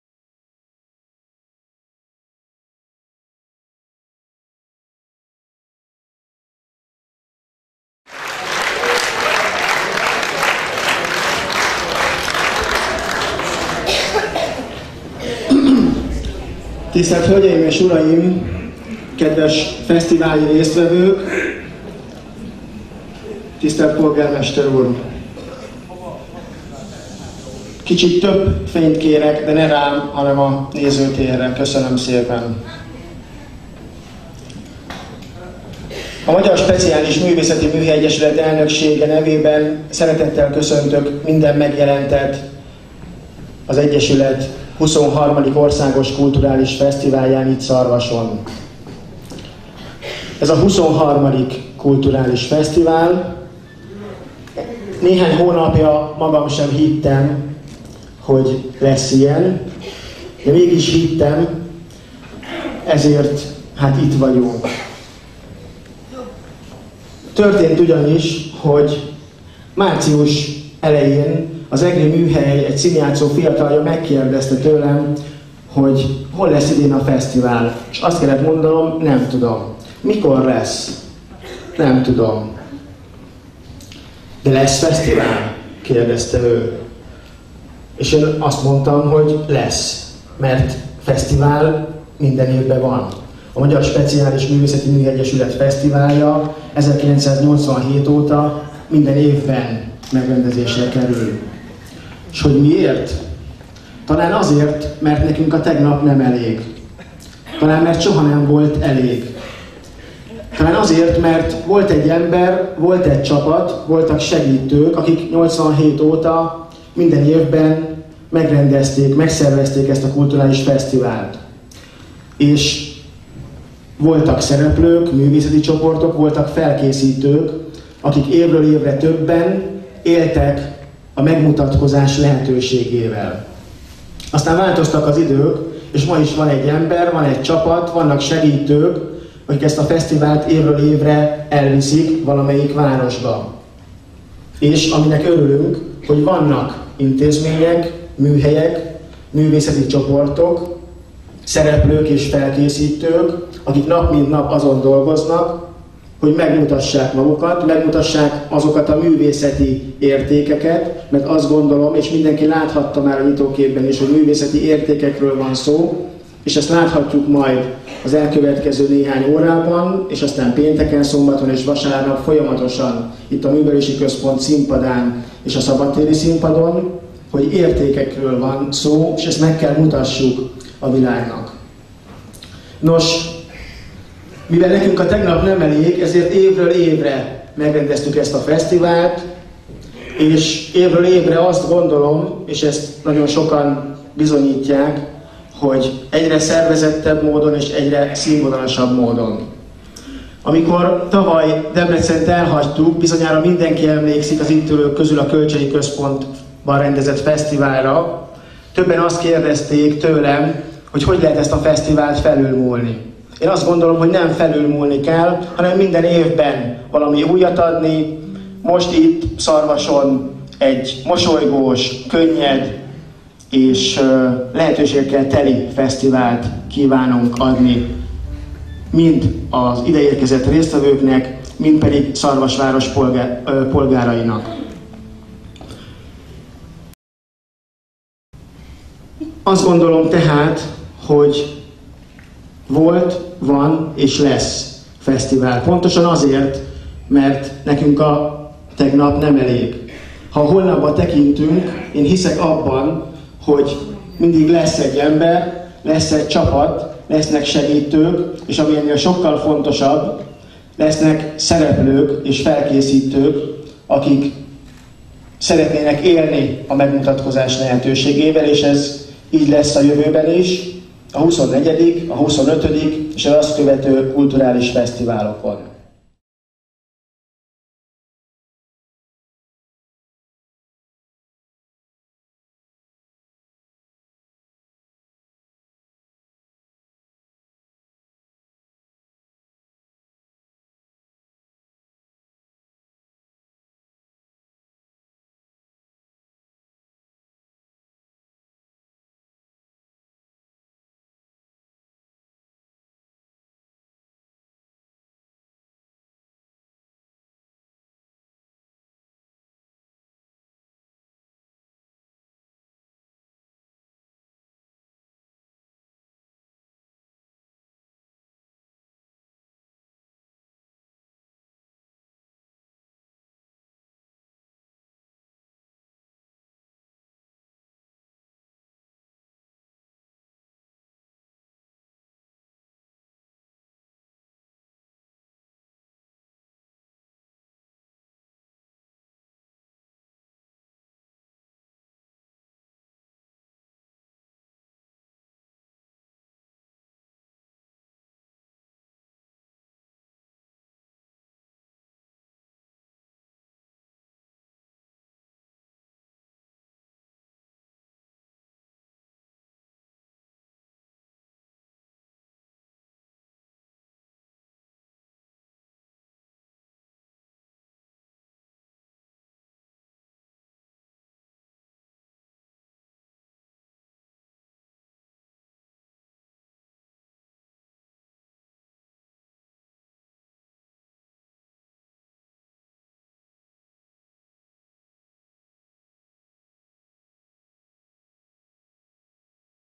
MSMME Asztalos Zsolt Elnök beszéde
A Magyar Speciális Művészeti Műhely Egyesület XXIII. Országos Kulturális Fesztiváljának megnyitóját nézhetitek meg kisfilmünkön! Főszerepben Asztalos Zsolt, elnök! :)